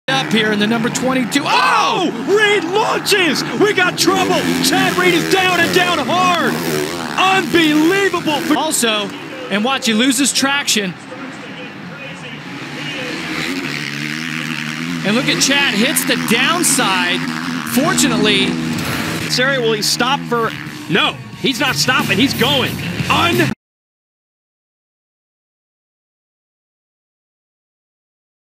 Chad Reed CRASH over the finish line jump.
Chad Reed MASSIVE flight over the finish line jump was lucky to land on the down slope. supercross monsterenergy monsterenergysupercross monsterenergyamasupercrosschampionship motocross dirtbike @billelandomedia406#cr22